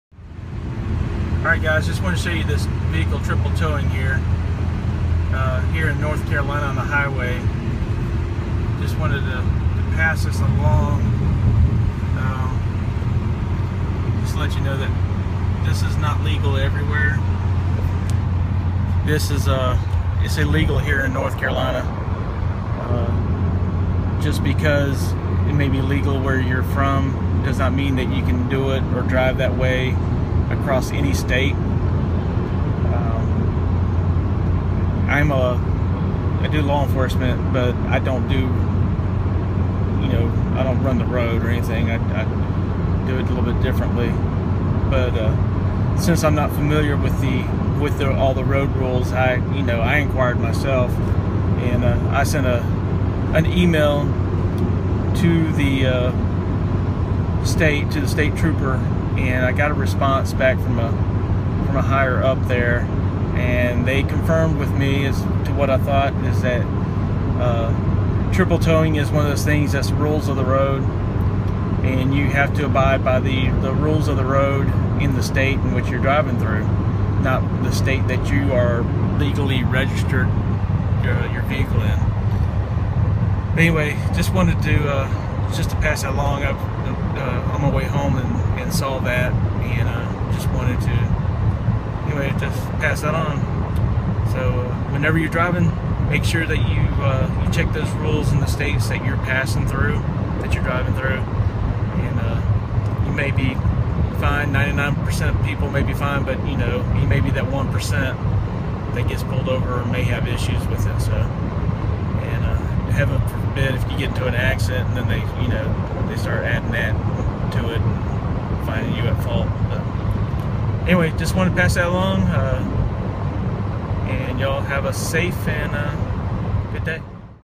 Double/Triple Towing - Check the rules of the road - Read Description!
When towing an even and you decide to double/triple tow, be sure to check he rules of the states in which you will be driving through. You have to follow the rules of the states your driving through and not just the state your vehicle is registered. Don't get hung up on "double" or "triple"...means the same thing in the RV world.

UPDATE: May 23, 2017. I know that a lot of viewers are not liking this and some who are truck drivers say it's ok. I was not able to find previous email from the North Carolina State Highway Patrol. So I emailed them again and here is the response I got. Sorry guys, same thing.

"Your email was forwarded to me in regards to your question about double and triple towing with an RV and the legalities of it in NC.  I understand it may be difficult to find when researching but I will try and point you in the right direction.  To explain this law there are a couple of different sections we have to look at in Chapter 20 (NC Motor Vehicle Law).

We need to first look at G.S. 20-116(e) (Size of vehicles and loads) It states “Except as provided by 20-115.1, no combination of vehicles coupled together shall consist of more than two units and no such combination of vehicles shall exceed a total length of 60 feet…”

Then we would need to look at 20-115.1 to see what the exception is being referred to in 20-116(e).  G.S. 20-115.1 (Limitations on tandem trailers and semitrailers on certain North Carolina highways.)   This statute explains in the first part that “Motor vehicle combinations consisting of a truck tractor and two trailing units may be operated in North Carolina only on highways of the interstate system and on those sections of the federal-aid primary system designated by the United States Secretary of Transportation.”  This statute continues on and addresses trailer lengths and other criteria but as far as it concerns with RVs we need to look at the definition of a truck tractor.

G.S. 20-4.01 (Definitions) defines a truck tractor as “Vehicles designed and used primarily for drawing other vehicles and not so constructed as to carry any load independent of the vehicle so drawn.”  By this definition an RV or pickup pulling an RV fails to meet the requirements of the law to allow for double and triple towing.